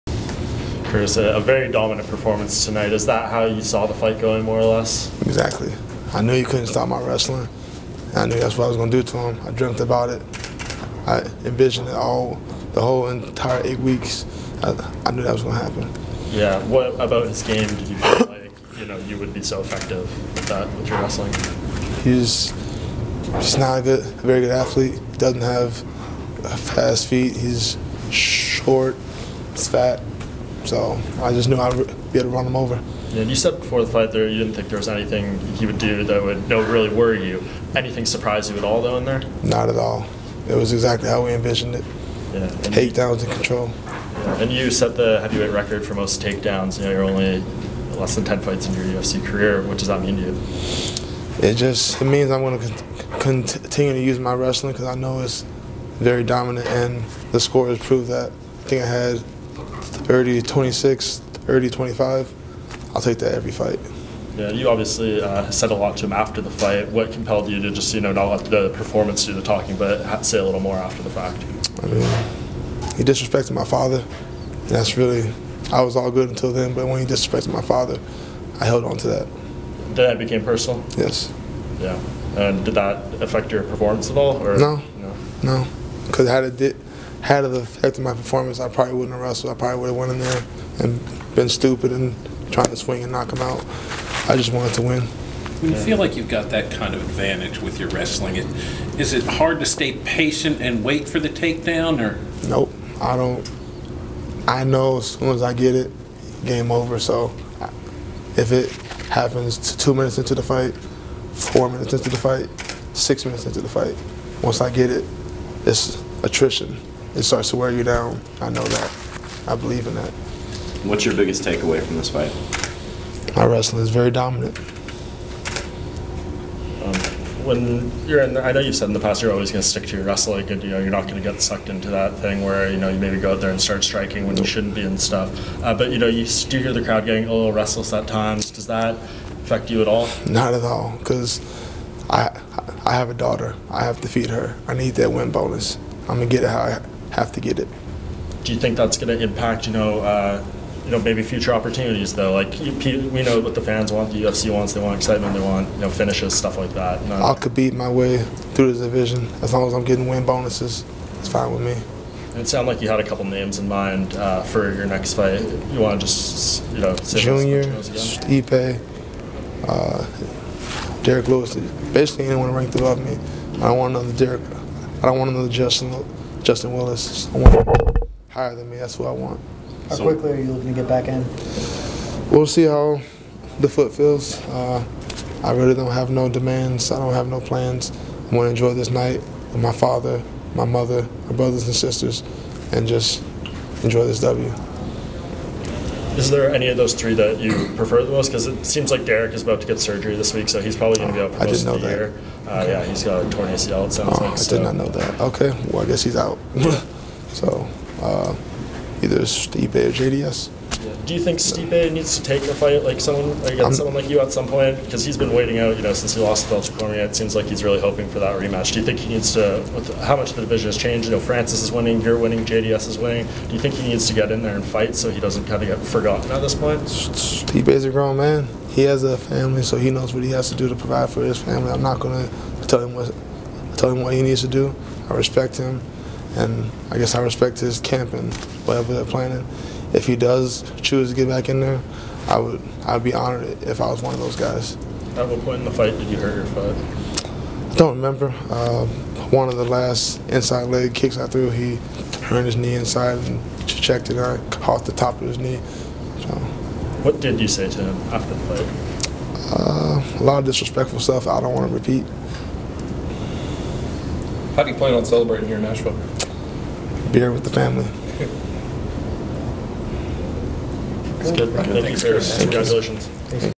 UFC Nashville: Curtis Blaydes post-fight interview
Curtis Blaydes speaks to the media following his UFC Nashville victory.